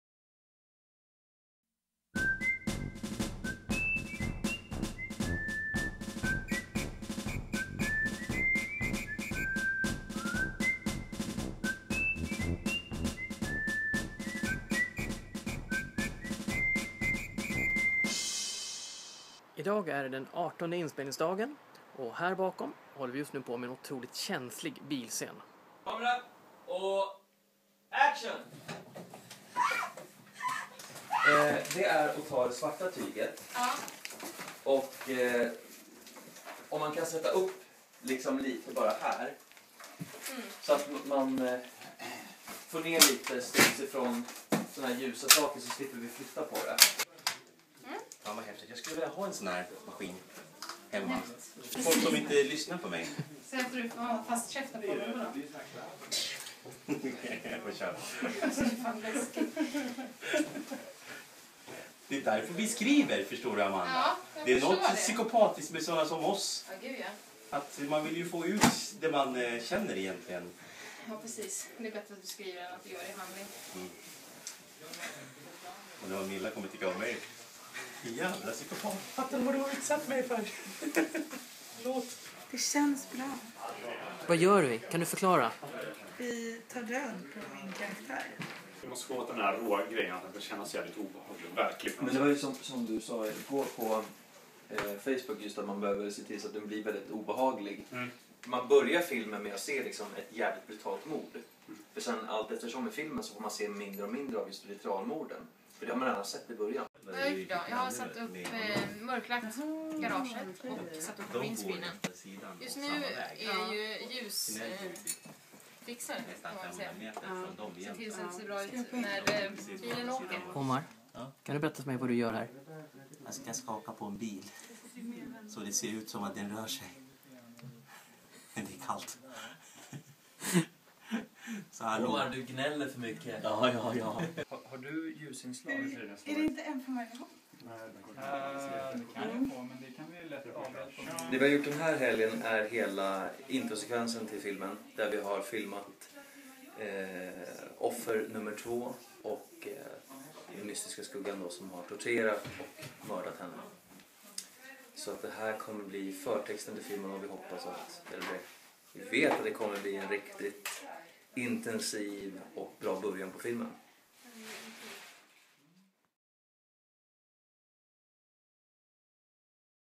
I fickan #19
Vi filmar det intensiva introt till filmen.